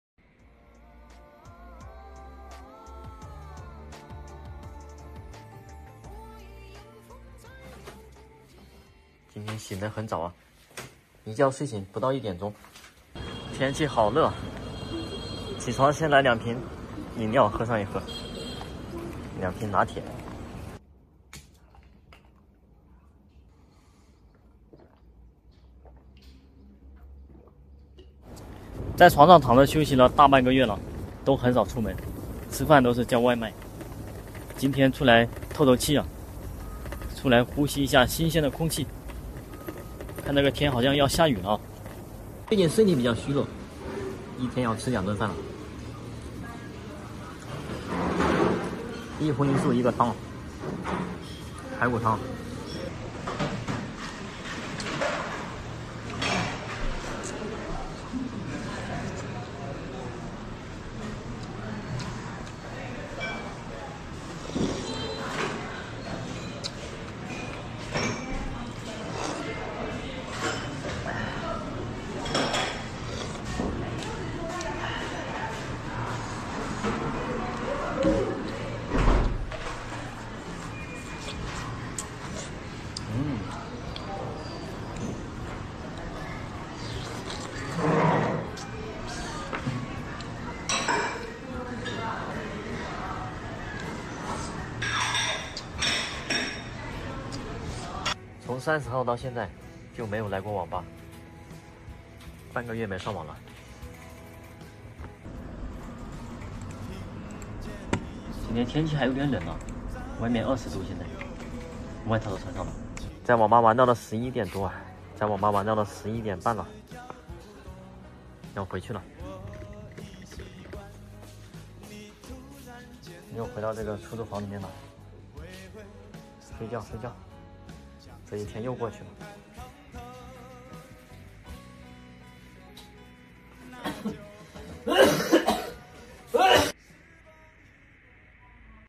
在床上躺了半个月了，天天吃外卖，今天出去呼吸一下新鲜的空气
人生,生活记录,记录